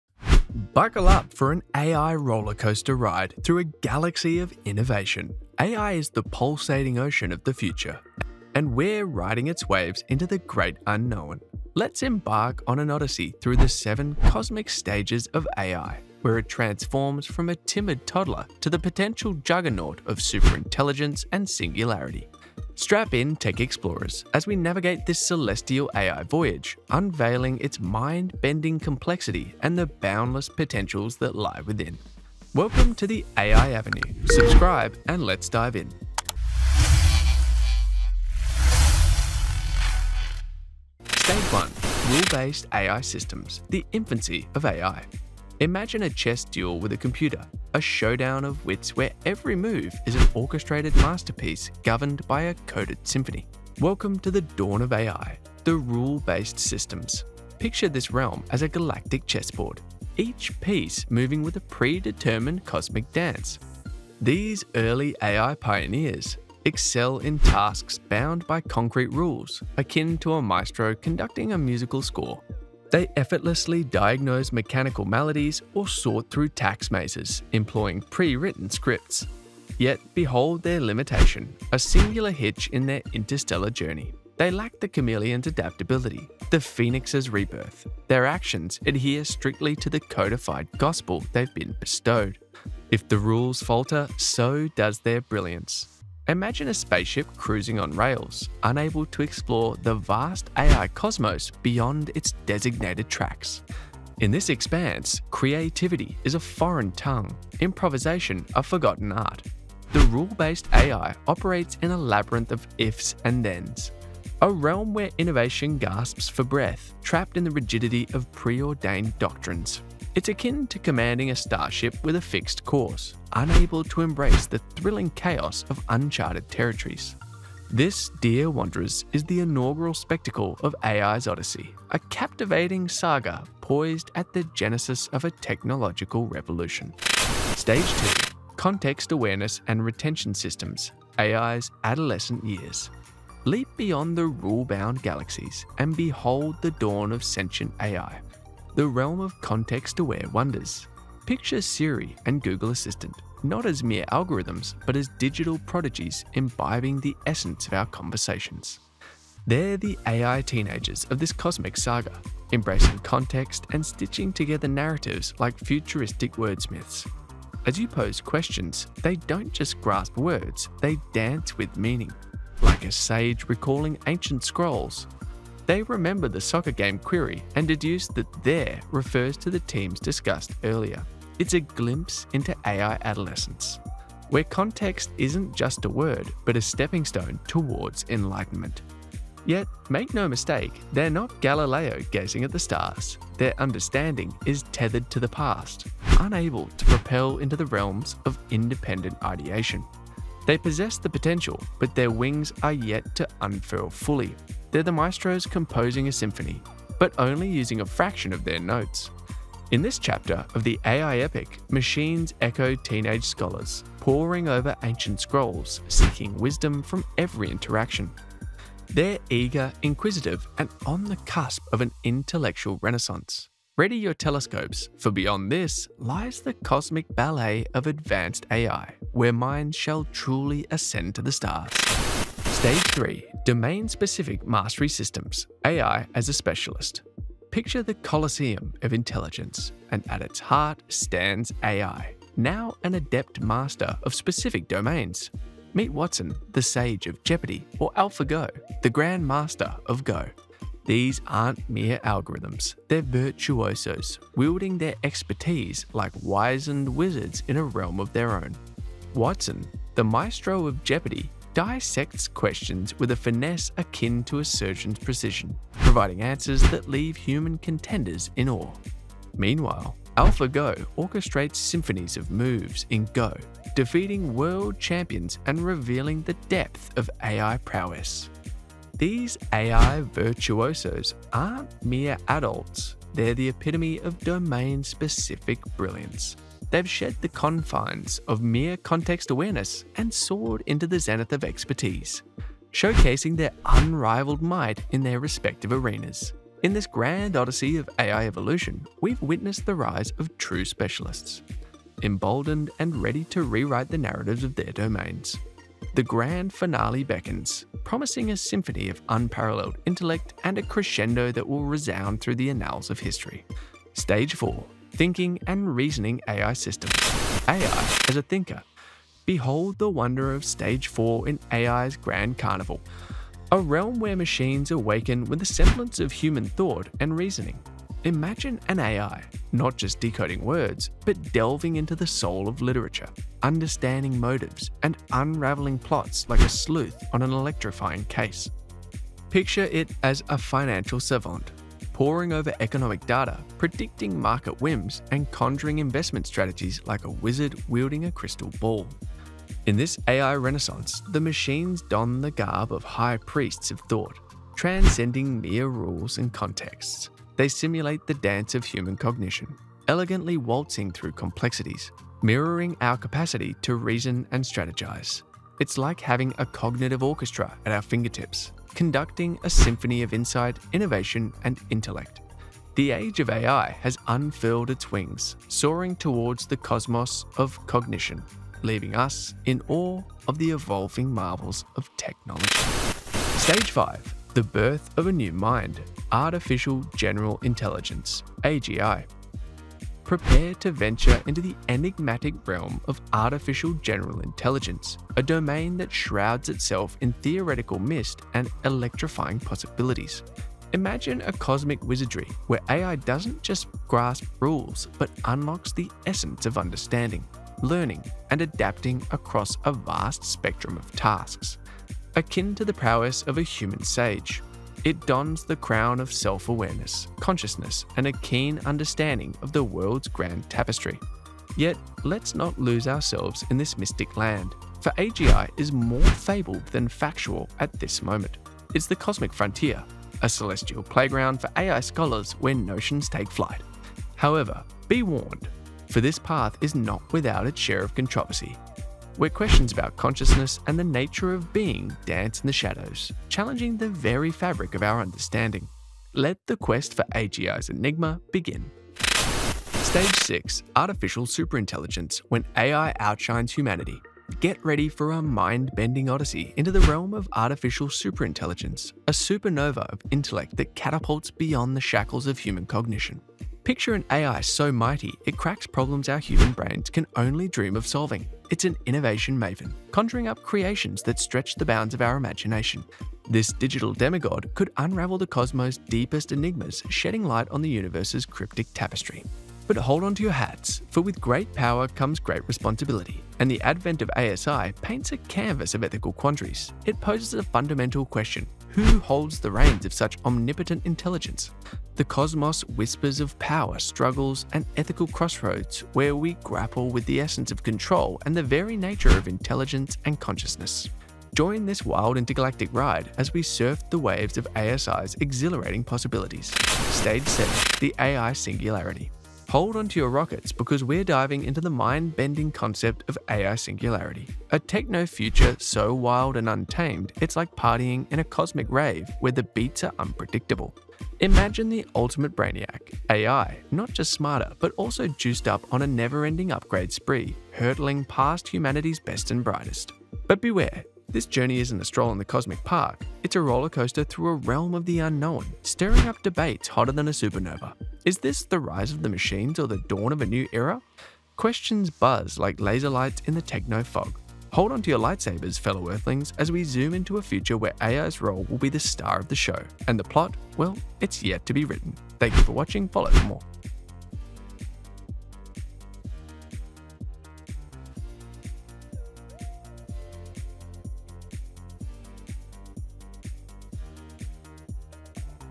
The 7 Stages of AI : Did you know?
Prepare to embark on a journey through the intricate layers of artificial intelligence! 🚀🧠 In this video, we're unveiling the mysteries of "AI Unveiled: Exploring the 7 Hidden Stages You Didn't Know About."

Artificial intelligence is not just a single leap into the future; it's a complex process with many stages. Join us as we delve deep into the world of AI and uncover the lesser-known phases that make it all possible.

In this video, we'll break down the seven hidden stages of AI, from data collection and preprocessing to algorithm development, training, and deployment. Discover how these stages work together seamlessly to bring AI-powered solutions to life.

Learn about the role of machine learning and deep learning in creating intelligent systems. Explore the challenges and innovations that occur behind the scenes, from fine-tuning models to handling real-world data.

But it's not just about the technical aspects; we'll discuss the ethical considerations of AI development, the impact on industries, and the future of artificial intelligence as it continues to evolve.

Whether you're a tech enthusiast, a future AI developer, or someone curious about the inner workings of this transformative technology, this video promises to provide you with a comprehensive understanding of AI's complexity and potential.

If you're ready to explore "AI Unveiled: Exploring the 7 Hidden Stages You Didn't Know About" and gain insights into the multifaceted world of artificial intelligence, hit that play button and prepare to journey through the stages that power the AI revolution. Join us in unveiling the secrets of AI!